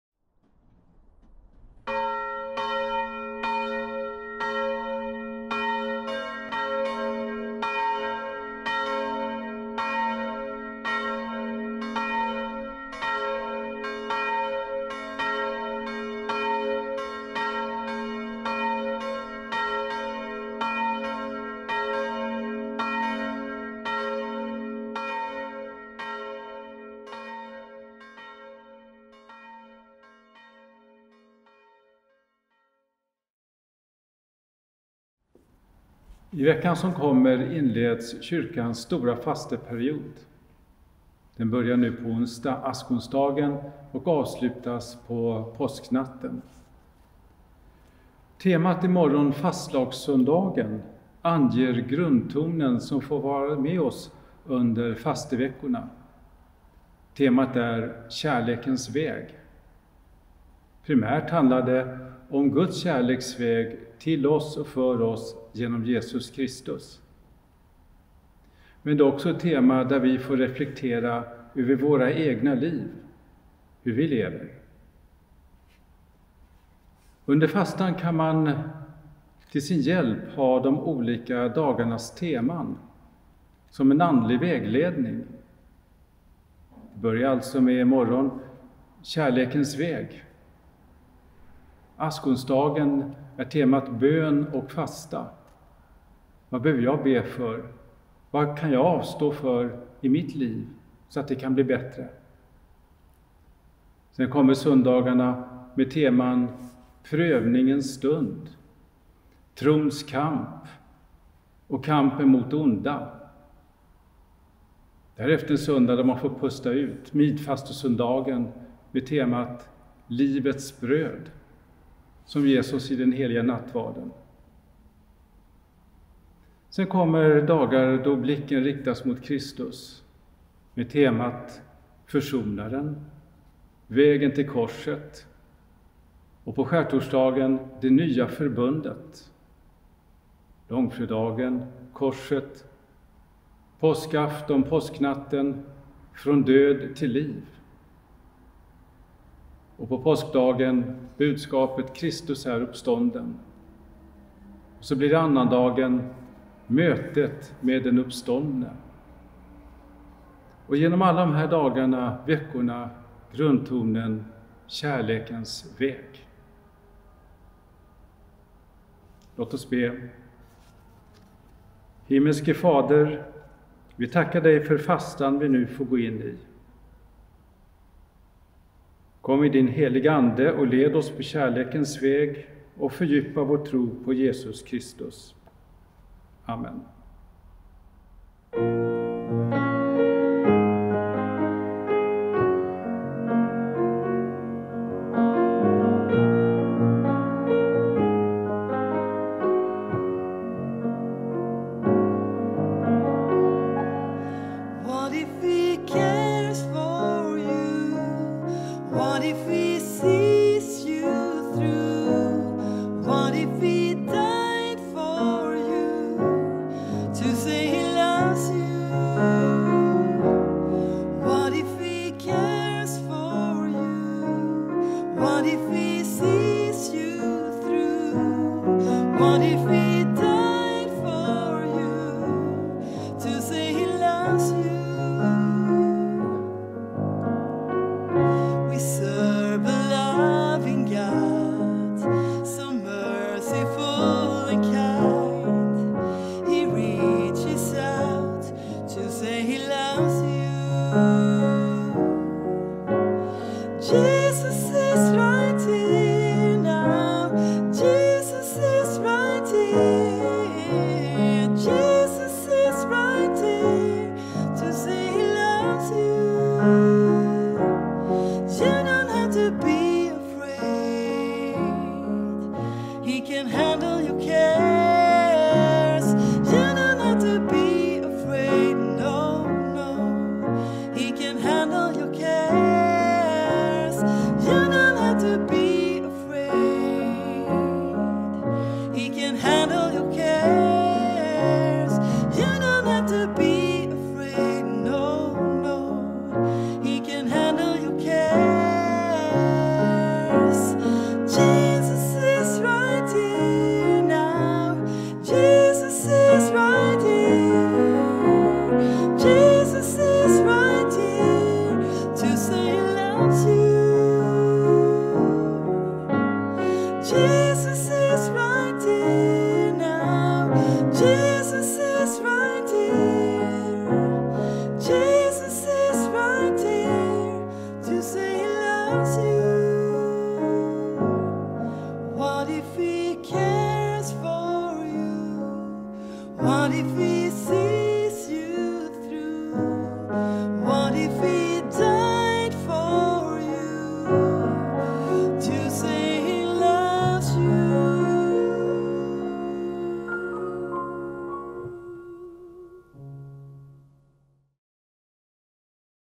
Helgmålsbön från Årsta kyrka 13 februari
Helgmålsbön från Årsta kyrka. Medverkande: Henrik Fergin och Camilla Stenman